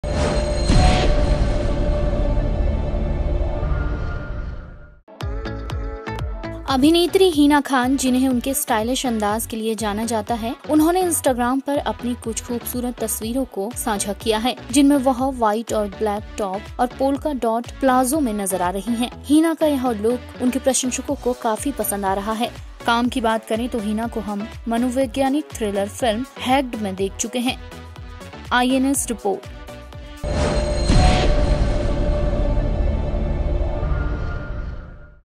स्टाइलिश अंदाज में नजर आई Hina Khan
अभिनेत्री हिना खान जिन्हें उनके स्टाइलिश अंदाज के लिए जाना जाता है उन्होने इंस्टाग्राम पर अपनी कुछ खूबसूरत तस्वीरों को साझा किया है।